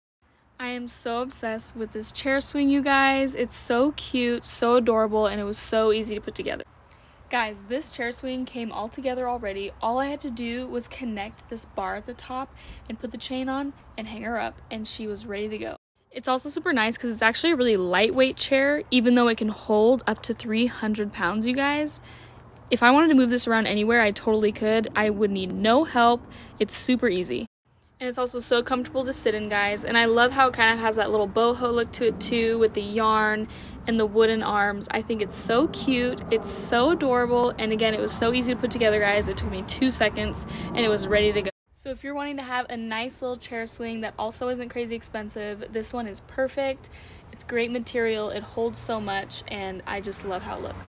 ROOITY Hammock Chair Hanging Swing with Wooden Armrests - Amazon
I love this hanging swing chair, it is so comfy and I just love the style of it. This chair can hold up to 300 LBS and they have different colors and designs that you can choose from. I love the boho look it has and the woven chair design makes it super comfy and stretchy!